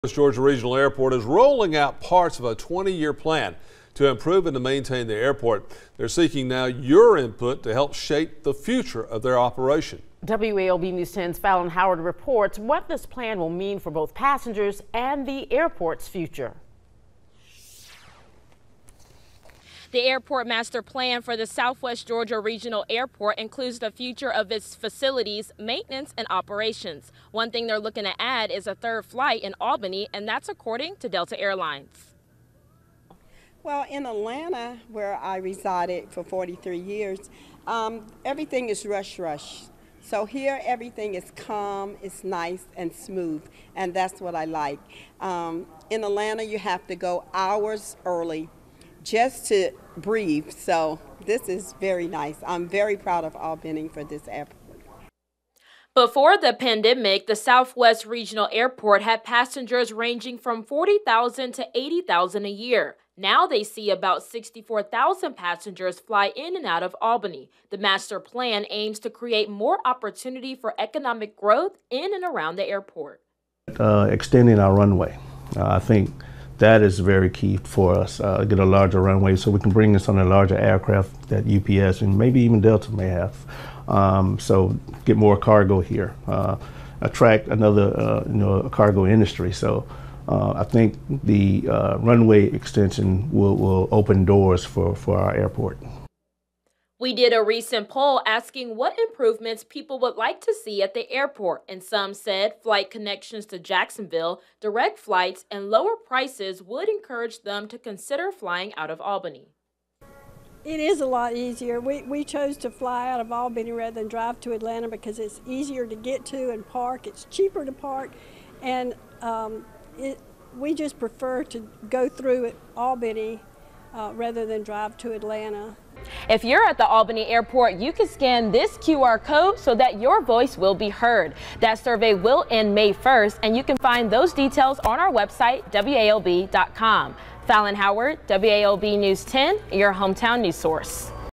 South Georgia Regional Airport asking for public’s input for Airpoort Master Plan
The 20-year plan will improve and maintain the airport.